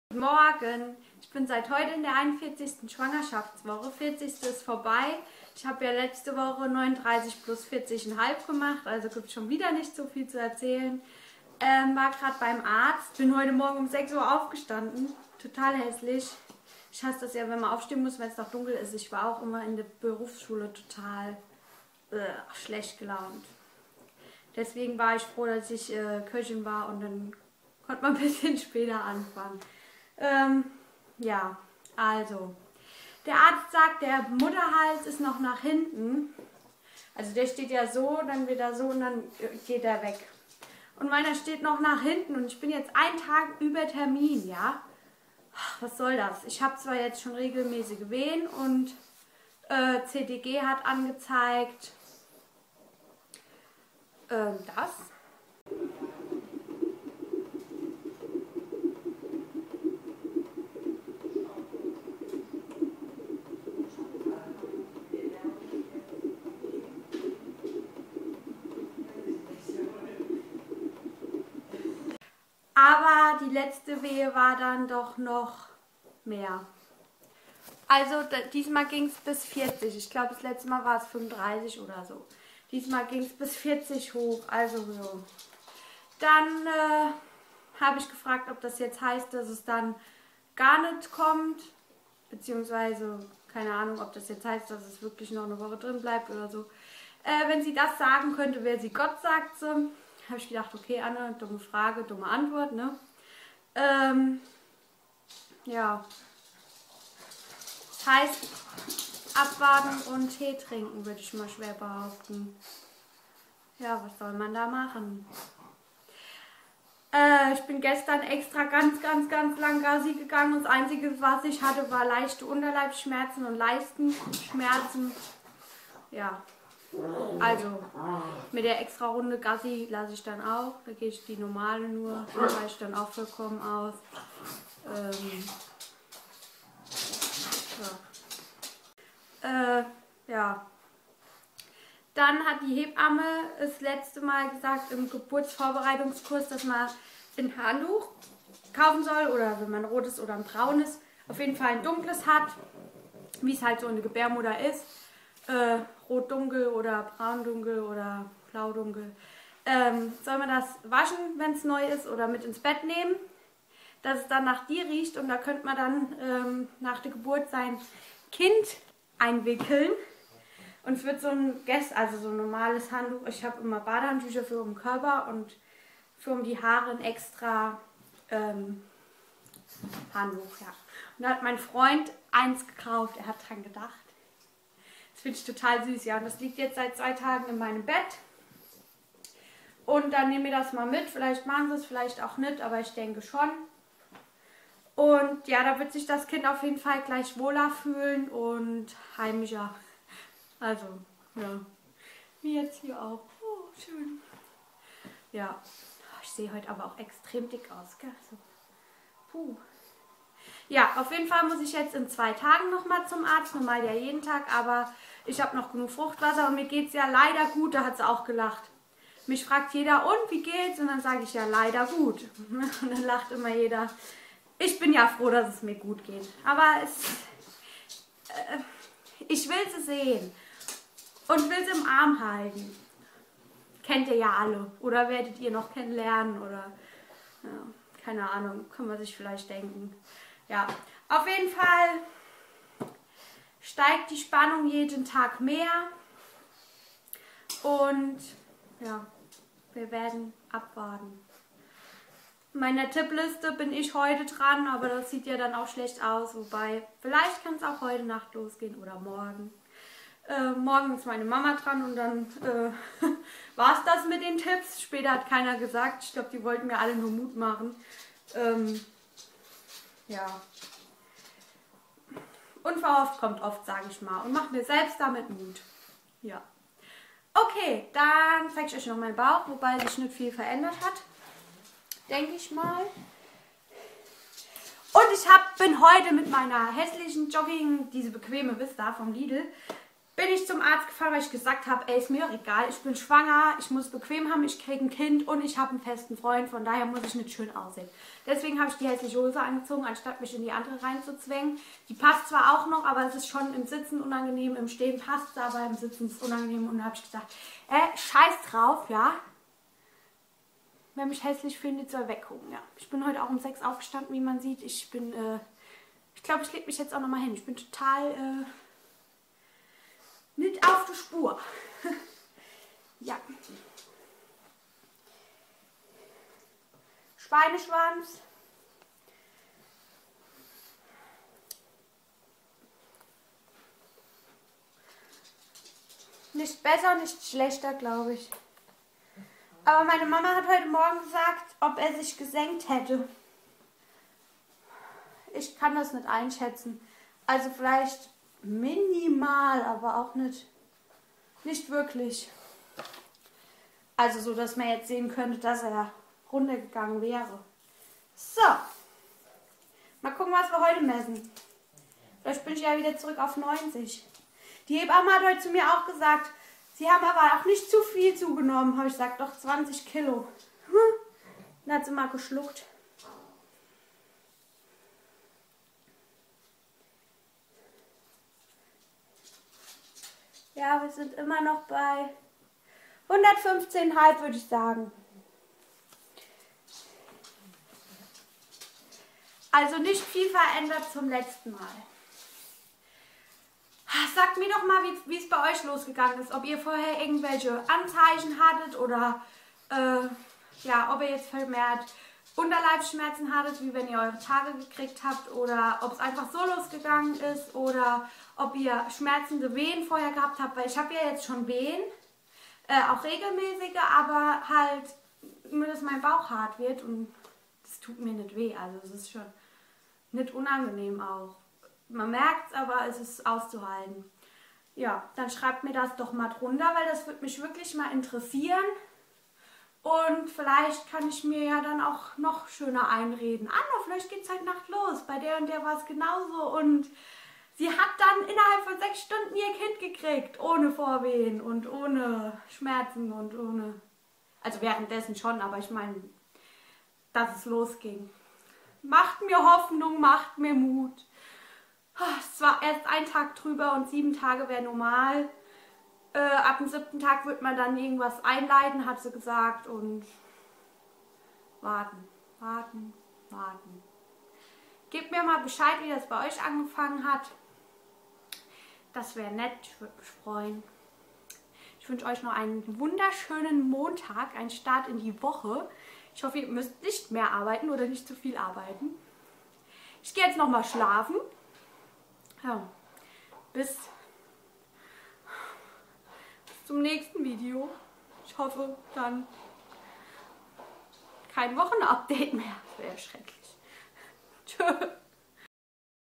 40 ssw abge
ja jetzt habe auch ich es geschafft ich bin drüber ;-(
mal sehen wann sich jo auf den weg macht ich bin gespannt ;-)
ich bei facebook ;-)